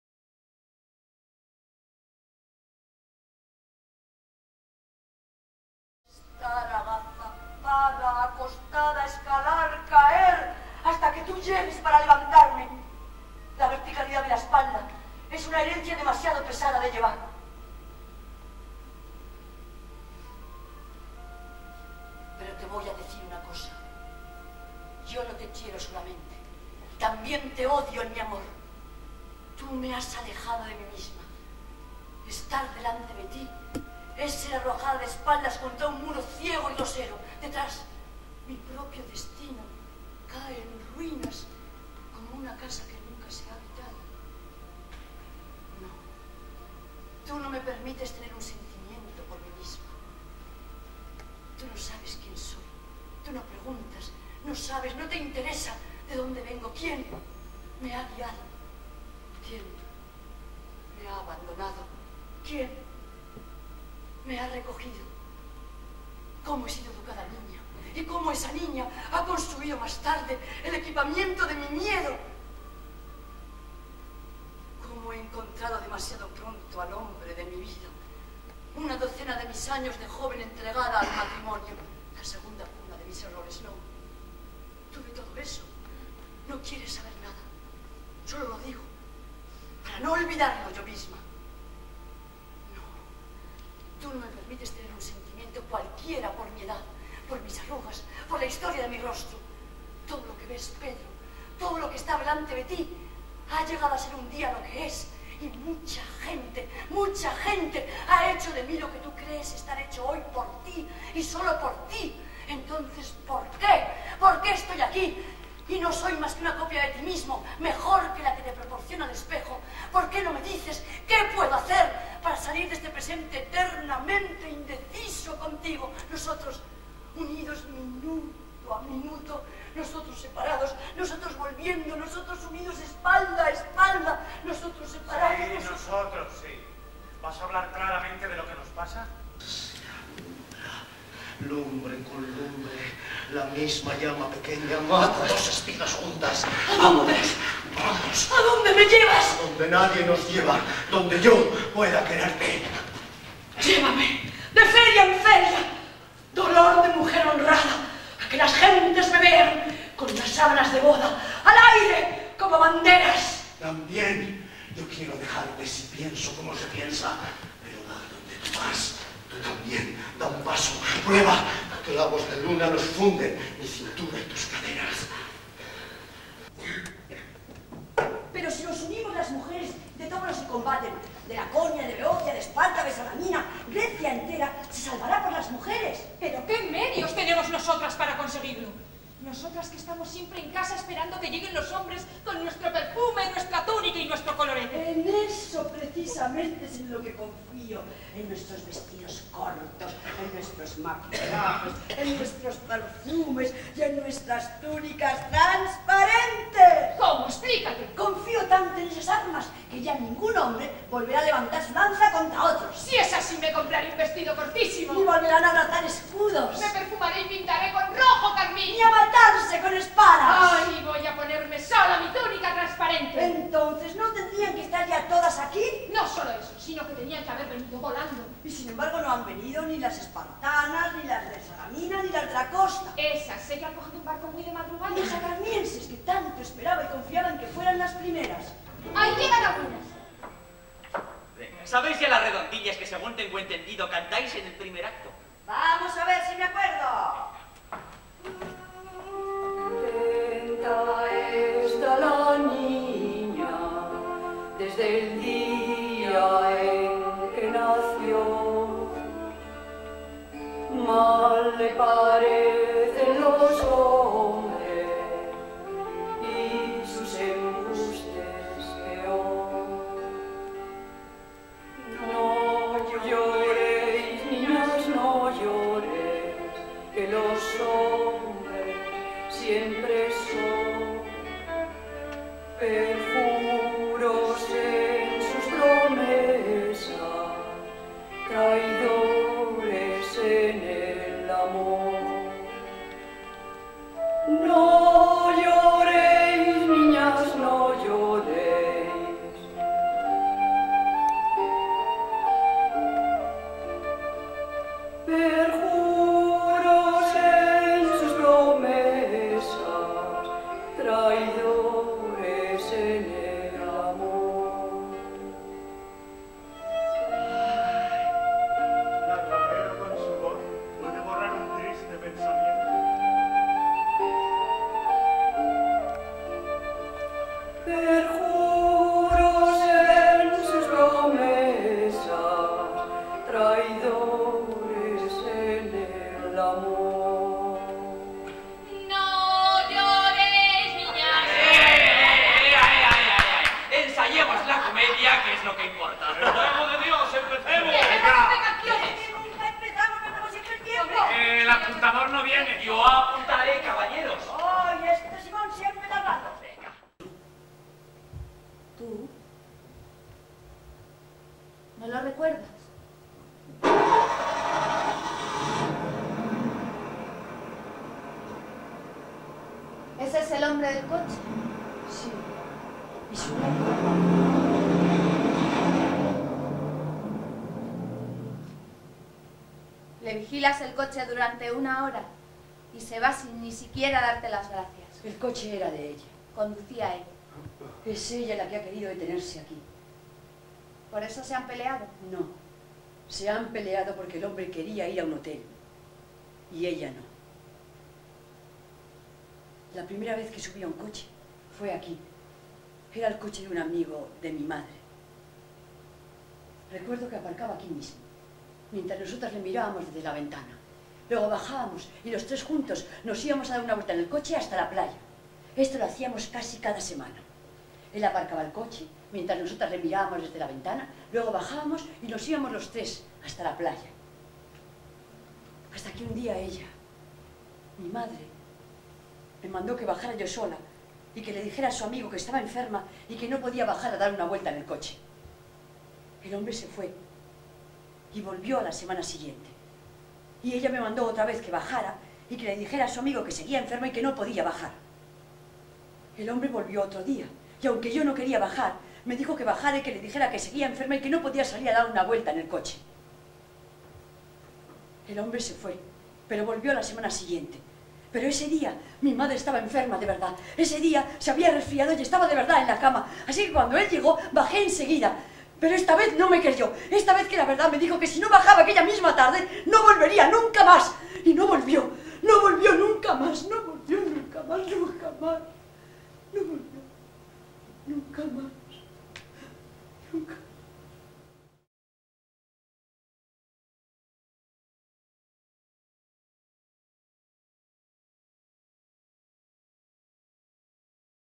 Homenaje a Pilar Laveaga
Durante más de diez años fui el encargado de realizar los vídeos de las obras que estrenaba el Teatro de la Ribera. Sirva este montaje con fragmentos de algunas de las obras como homenaje a su directora, Pilar Laveaga.